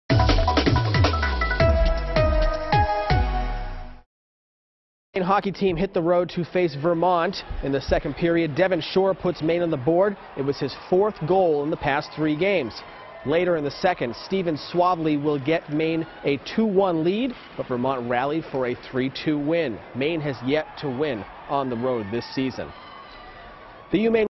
UMaine hockey loses at Vermont
The Black Bear remain winless on the road this season.